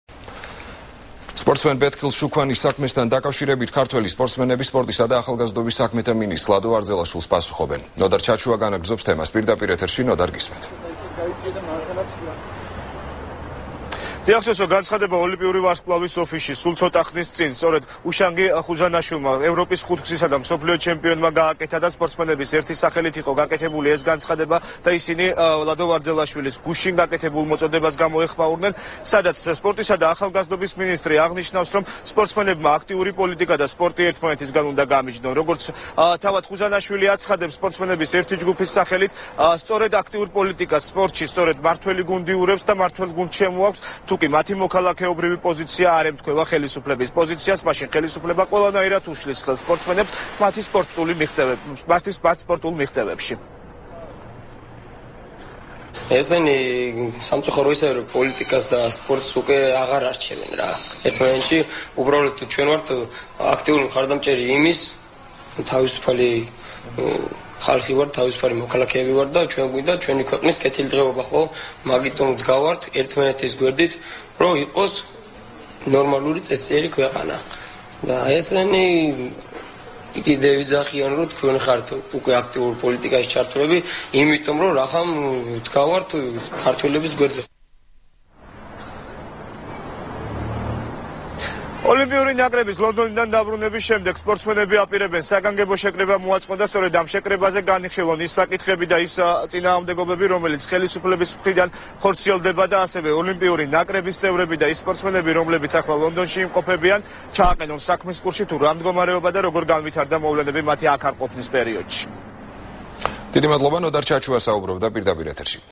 ახალი 12 | სპორტსმენების პასუხი | 03.08.12
ბეთქილ შუკვანის საქმესთან დაკავშირებით ქართველი სპორტსმენები სპორტისა და ახალგაზრდობის საქმეთა მინისტრს ლადო ვარძელაშვილს პასუხობენ.